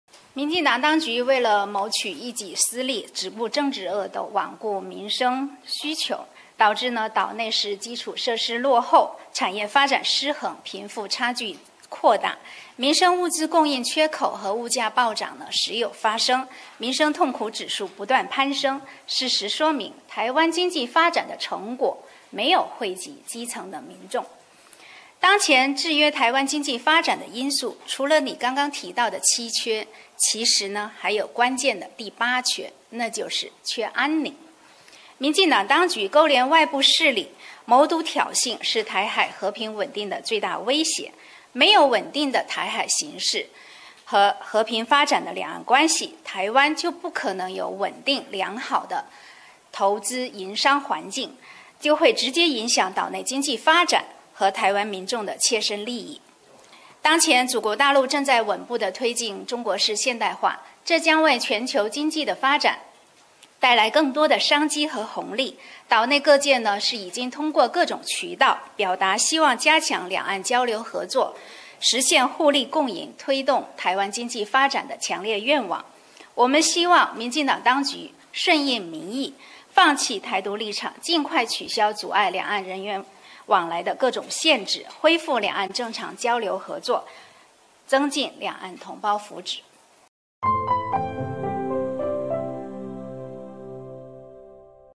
台湾岛内“缺水、缺电、缺地、缺工、缺人才” 国台办：民进党只顾政治恶斗 罔顾民生需求
4月12日，国台办举行例行新闻发布会。会上，有记者问，民进党当局上台七年来，不仅岛内“缺水、缺电、缺地、缺工、缺人才”问题未见好转，还再添“缺蛋、缺药”问题，引发岛内民怨。请问发言人有何评论？对此，国台办发言人朱凤莲表示，民进党当局为谋取一己私利，只顾政治恶斗、罔顾民生需求，导致岛内基础设施落后，产业发展失衡，贫富差距扩大，民生物资供应缺口和物价暴涨时有发生，民生痛苦指数不断攀升。事实说明，台湾经济发展成果没有惠及普通民众。